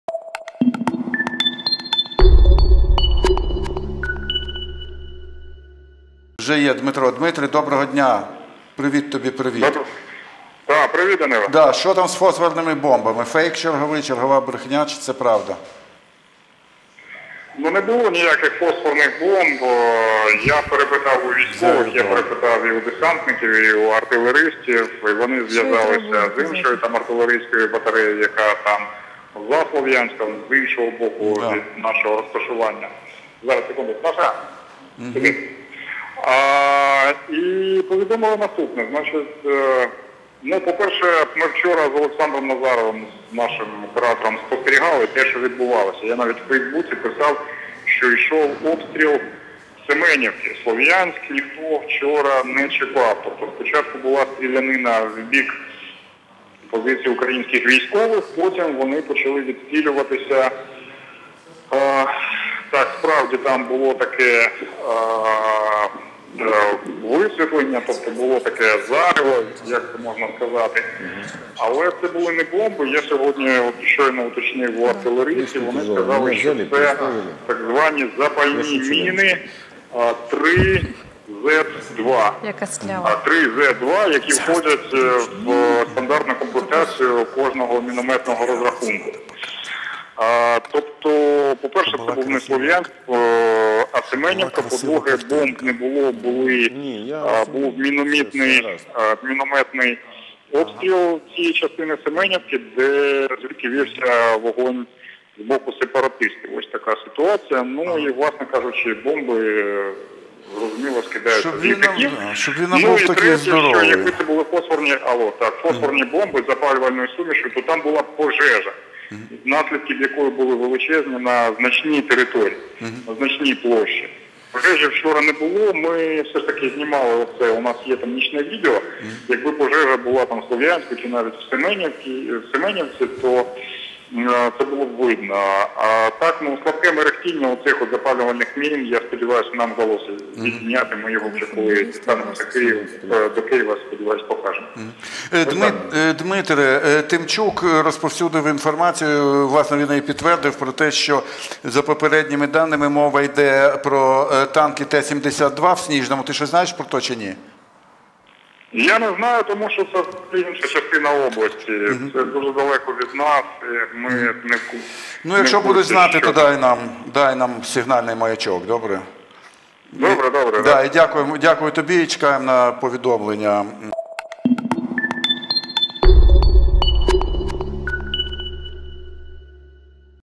Гнап: Семенівку обстрілювали стандартними мінами 3-З-2 , а не фосфорними бомбами
Журналіст Дмитро Гнап телефоном на Громадському 12.06.14